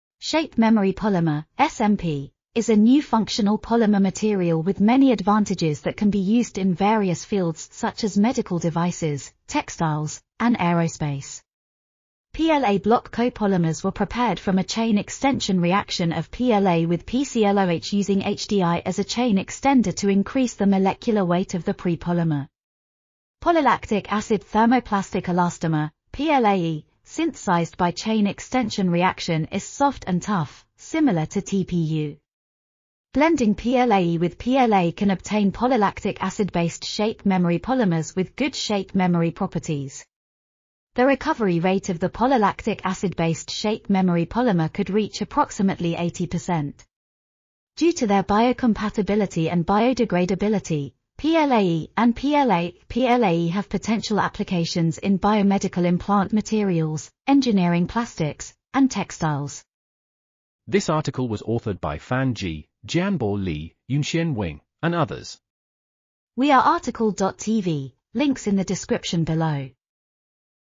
Synthesis of PLA-based thermoplastic elastomer and study on preparation and propertie... | RTCL.TV
### Keywords ###

### Article Attribution ###
Title: Synthesis of PLA-based thermoplastic elastomer and study on preparation and properties of PLA-based shape memory polymers
Authors: Fan Ji, Jianbo Li, Yunxuan Weng ,and Jie Ren
Publisher: IOP Publishing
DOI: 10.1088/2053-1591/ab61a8

### Video Timestamps ###
0:00:00 - Summary
0:01:00 - Title
0:01:06 - Tail
0:01:10 - End